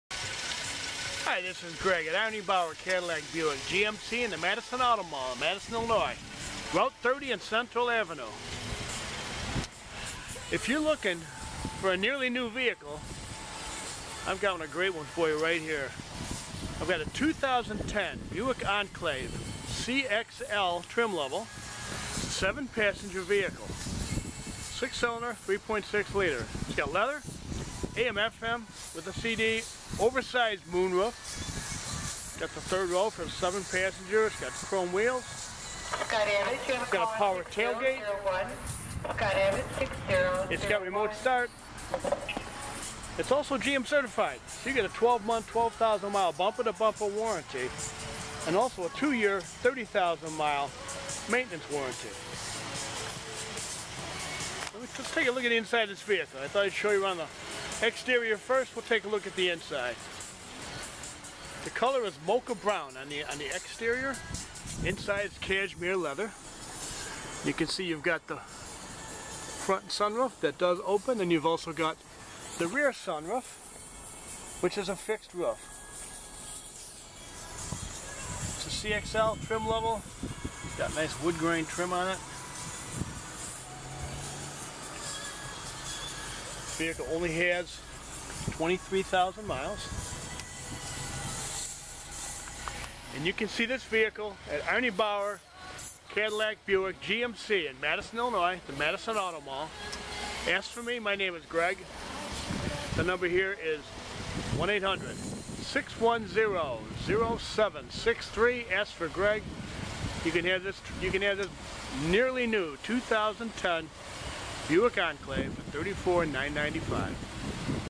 2010 Buick Enclave CXL! $34,995.! Arnie Bauer Cadillac/Buick/GMC
Side Impact Air Bag(s) Electronic Climate Control Heated Seat(s) Fog Lights Power Seat Air Conditioning Cooled Seats Bose Sound System Dual Air Bags Wood Grain Trim Pkg. Cruise Control On*Star System Power Windows Back Up Camera Leather Upholstery Power Moonroof Chrome Wheels 3rd Row Seats Tilt Steering Wheel Power Door Locks